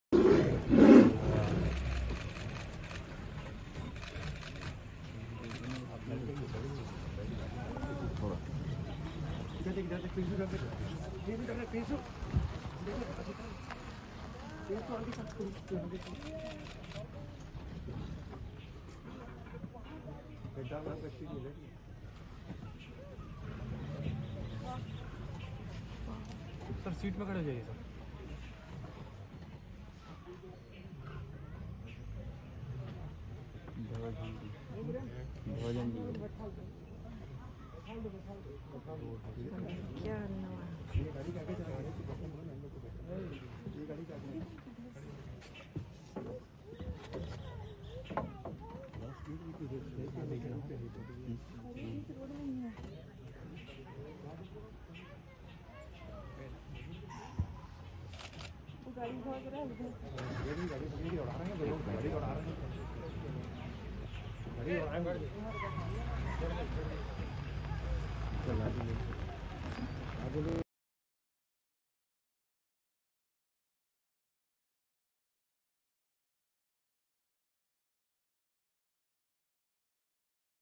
The jungle king is showing his kingdom to his beauty queen. Kanha National Park Madhya Pradesh India
Dj female tigress and DB3 Tiger walking together

This short video has been taken by Mr. Rohit Khairwar and he is a naturalist and driver in Kanha National Park